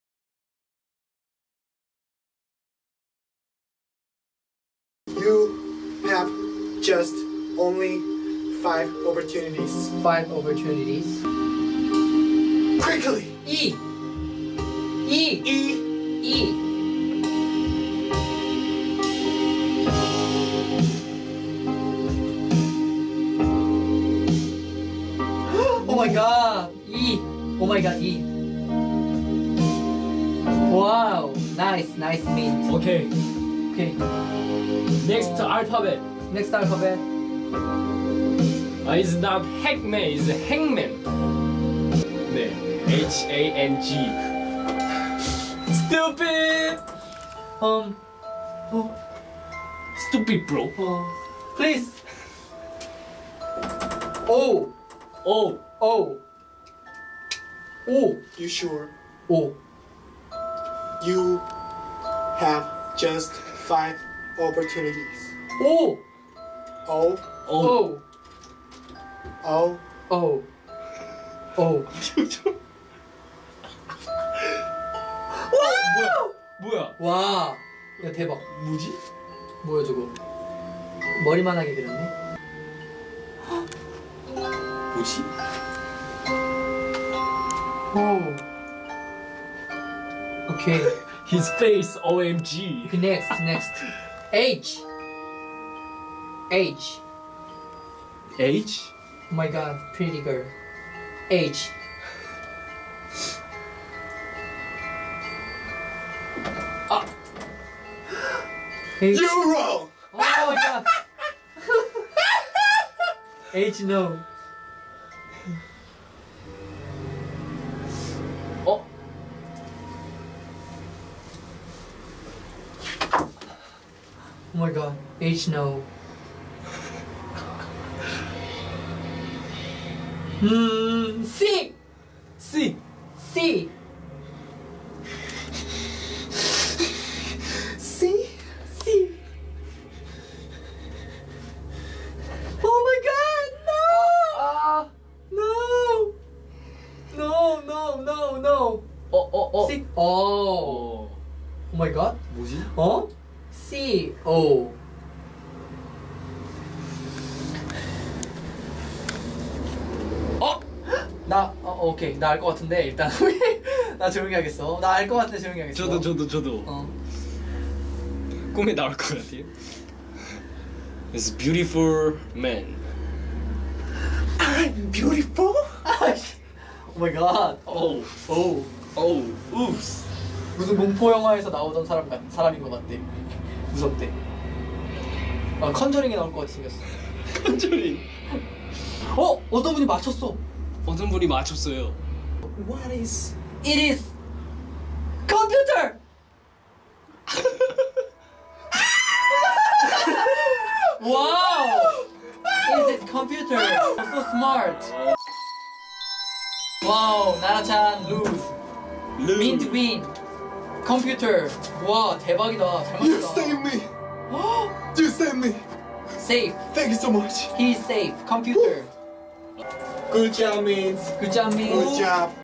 공포의 행맨게임 with 나라찬
나라찬의 공포 행맨게임!!
인스타 라이브 중 행맨게임을 하다
삘받은 나라찬에게 들린 행맨신!!

민트는 행맨신으로부터 나라찬을 구할 수 있을 것 인가?!